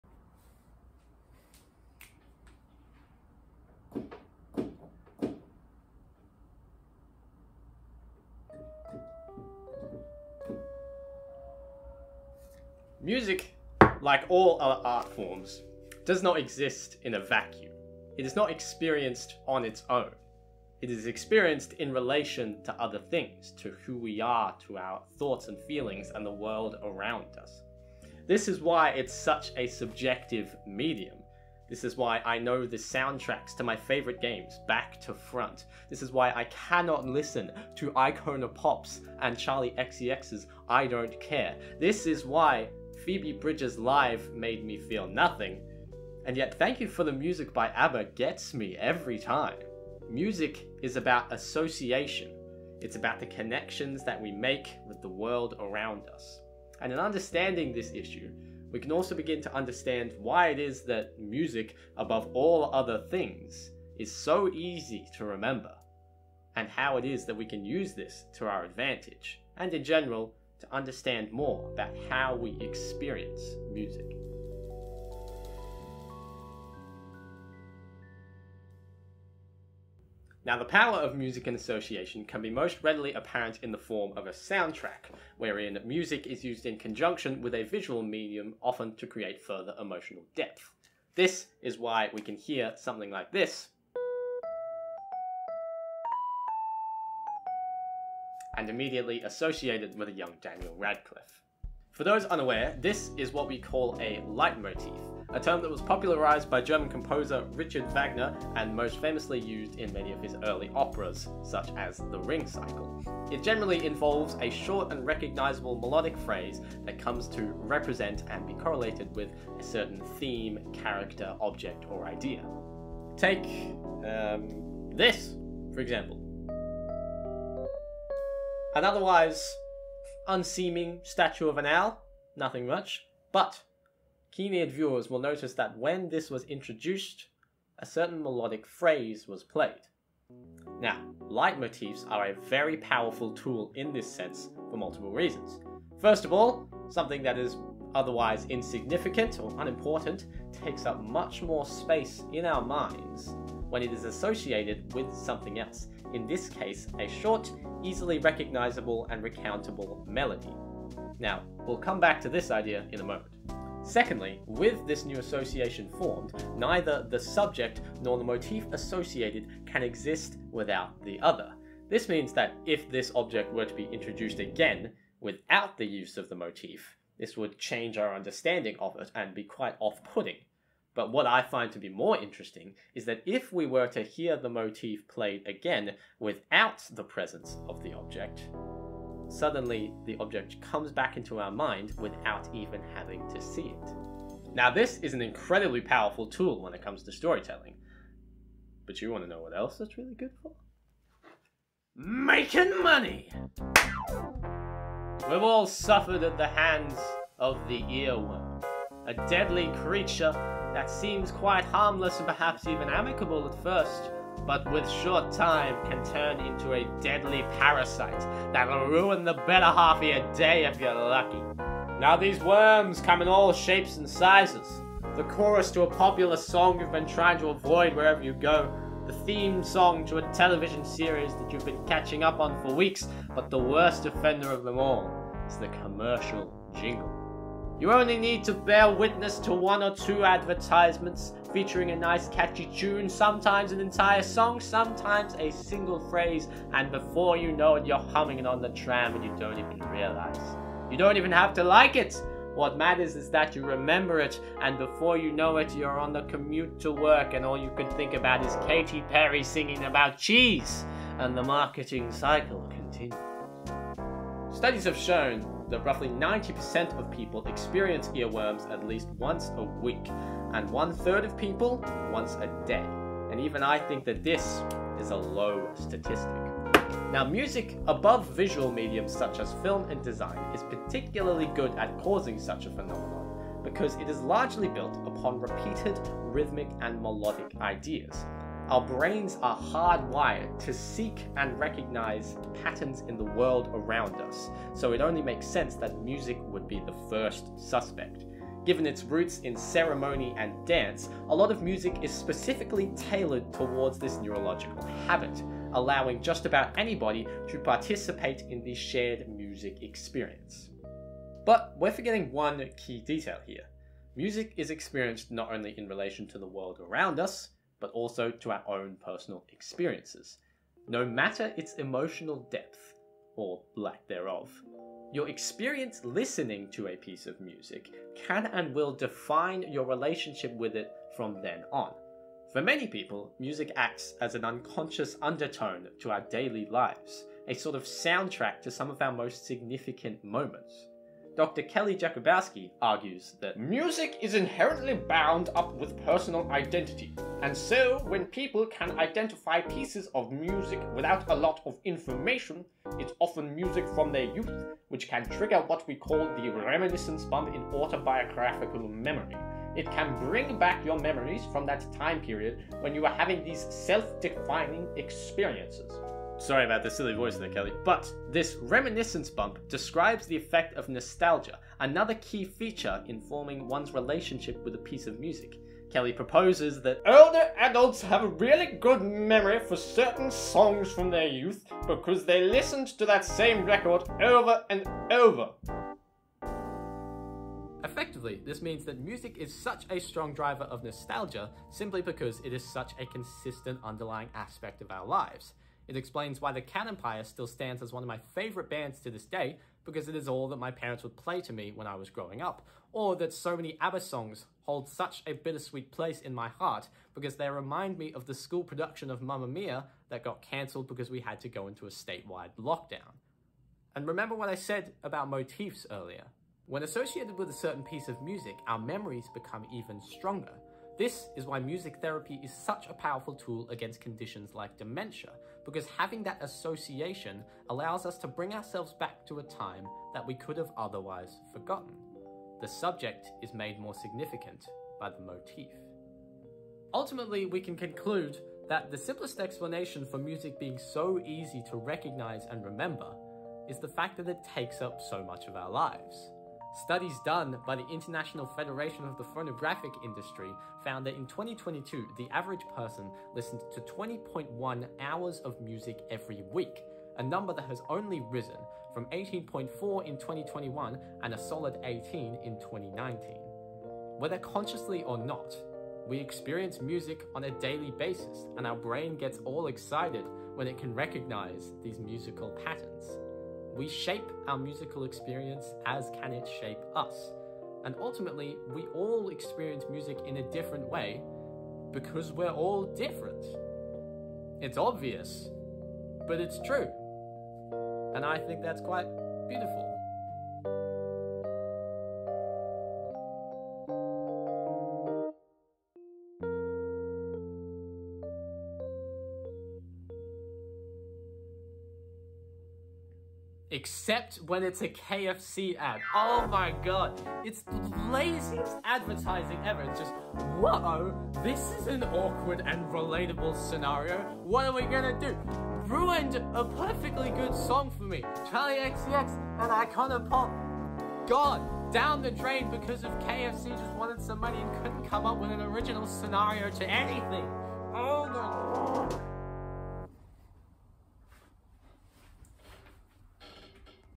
Music and the Mind
A silly ramble about music that I made for uni

Sources:
1 -

00:00 - Intro
01:20 - Motifs
03:18 - Earworms
05:11 - Nostalgia
07:16 - So???
08:23 - Addendum regarding KFC